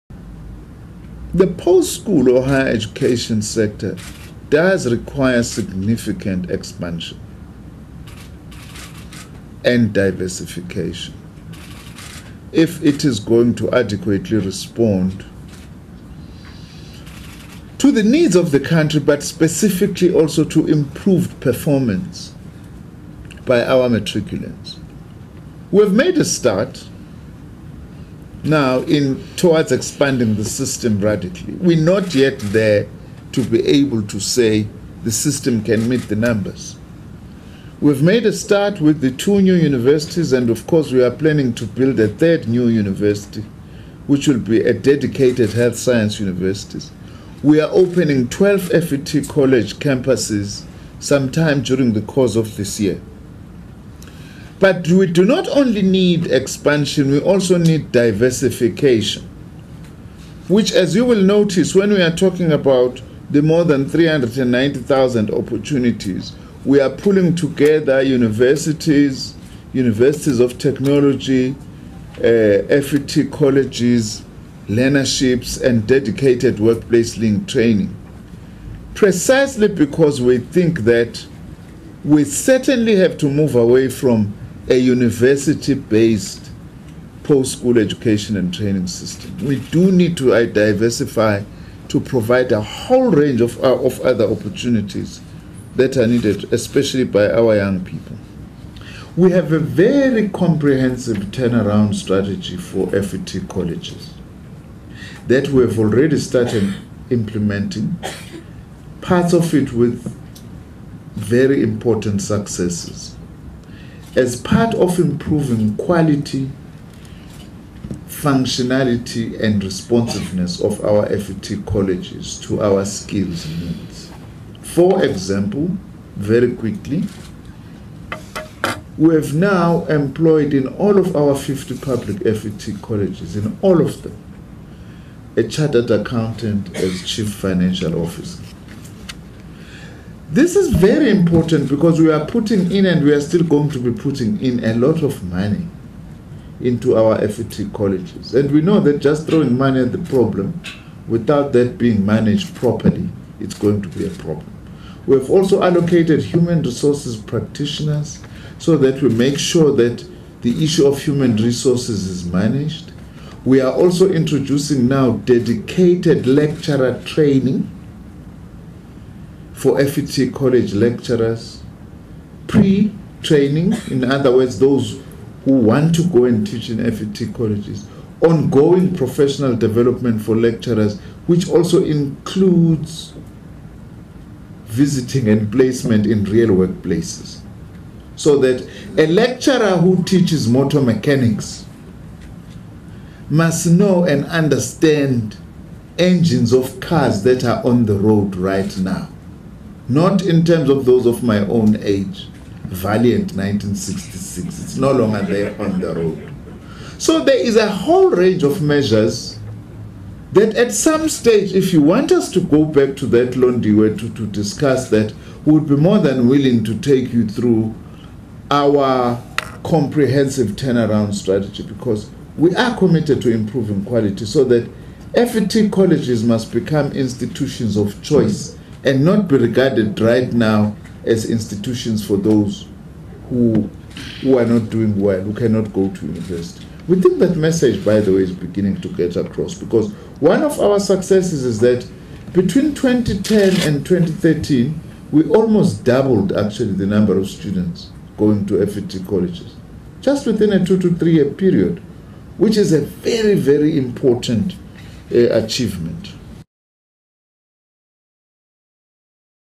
SA future lies in FET colleges -- Nzimande
South Africa had to move away from a university-based post school education and training system for its skills needs to be effectively met, Higher Education and Training Minister Dr Blade Nzimande said on Thursday.

Addressing the media in Pretoria, he said further education and training (FET) colleges had to be turned into institutions of choice, as "that is where the future of this country lies".